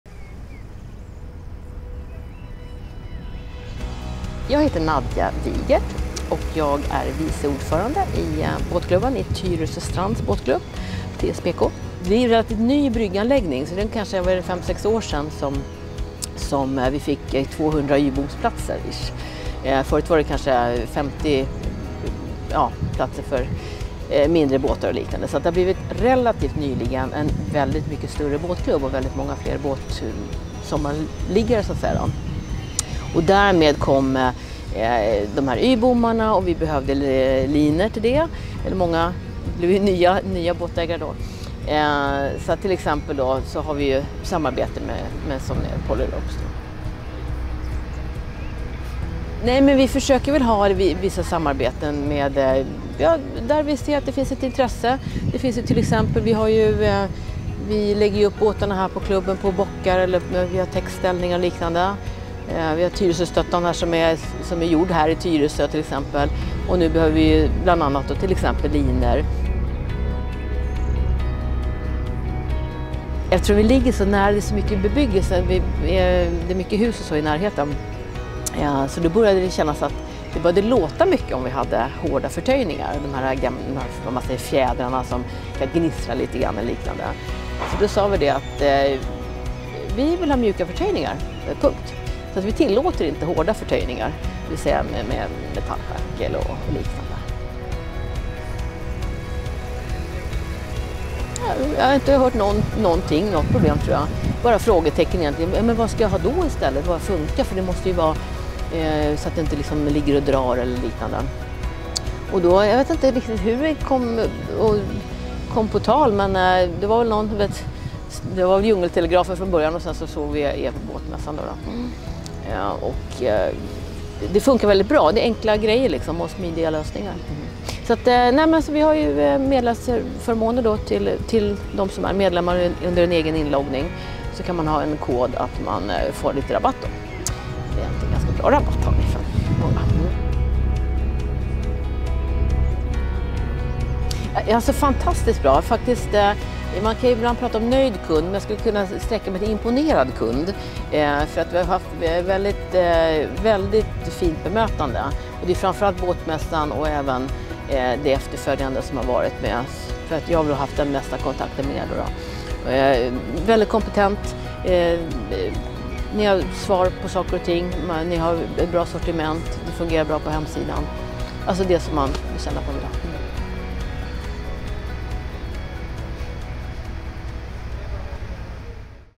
POLYROPES MÖTER TYRESÖ STRANDS BÅTKLUBB
PolyRopes åkte till Stockholm för att hälsa på hos Tyresö Strands Båtklubb. De har en regel i sin båtklubb som förbjuder "hårda förtöjningar", det vill säga förtöjningar med fjäder eller schaklar i metall. Genom ett samarbete med PolyRopes kan de erbjuda rabatt på alla PolyRopes förtöjningslinor till sina medlemmar.